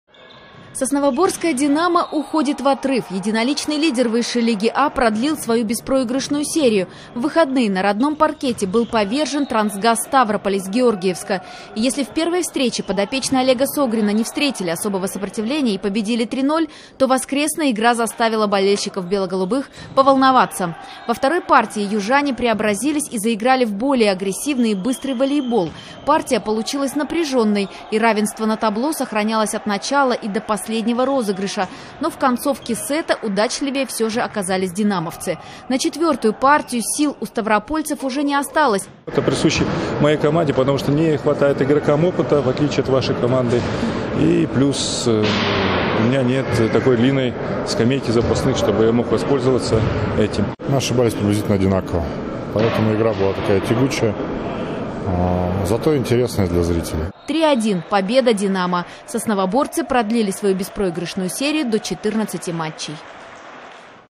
Сосновоборское "Динамо" продлило беспроигрышную серию
&nbsp;Сосновоборское &quot;Динамо&quot; уходит в отрыв. Единоличный лидер &nbsp;Высшей Лиги &laquo;А&raquo; продлил свою беспроигрышную серию.